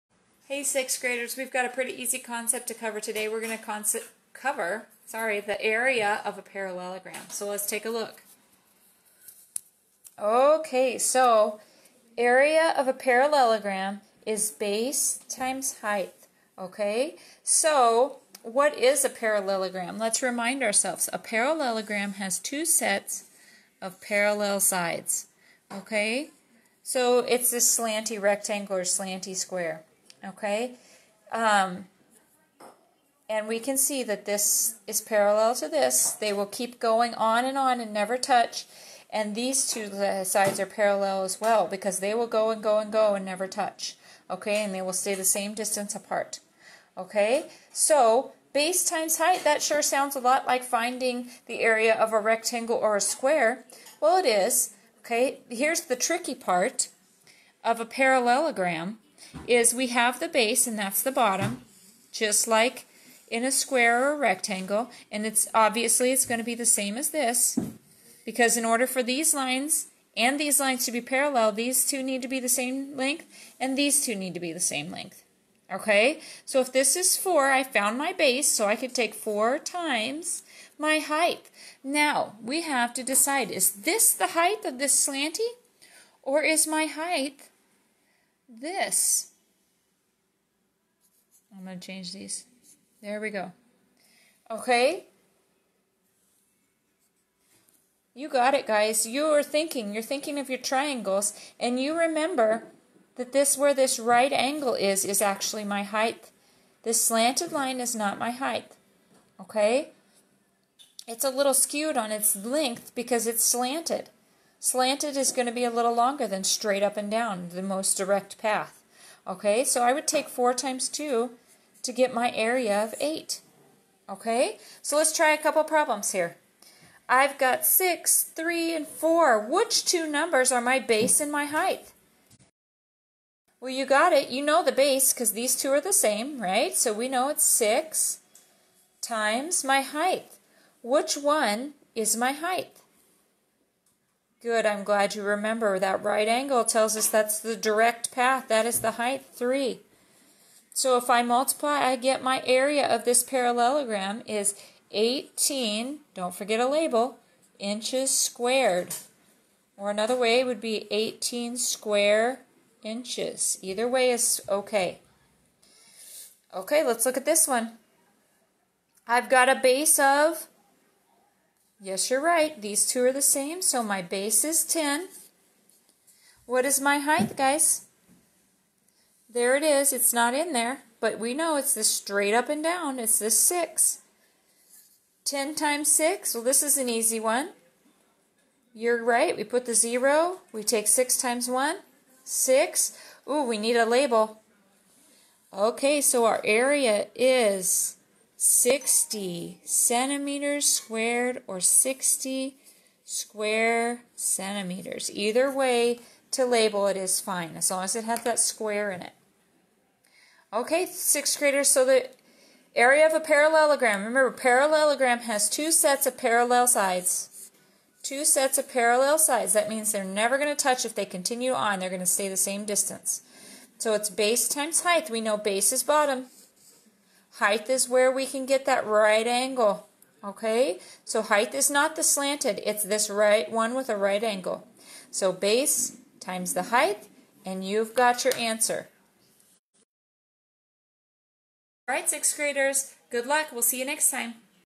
6th math area of a parallelogram
We will discuss and practice finding the area of a parallelogram